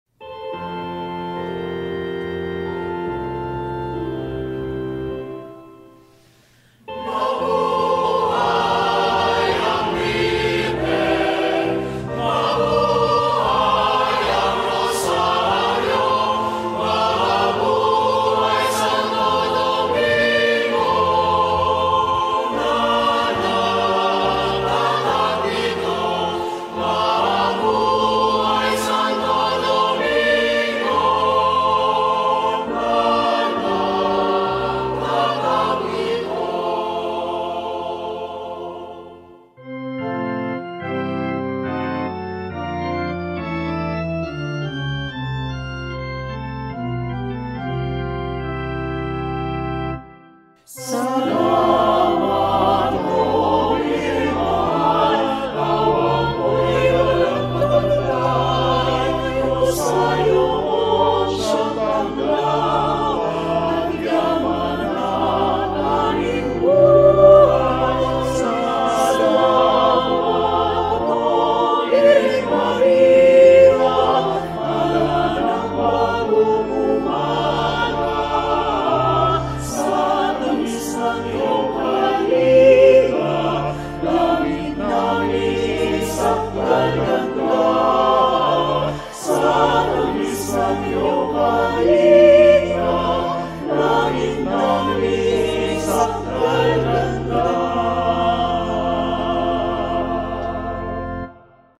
Scenes from the Solemn Procession of Our Lady of the Rosary - La Naval de Manila 2024
With 200,000 reported devotees in attendance, here are some scenes from the Solemn Procession of Nuestra Señora del Santisimo Rosario - La Naval de Manila last October 13, 2024.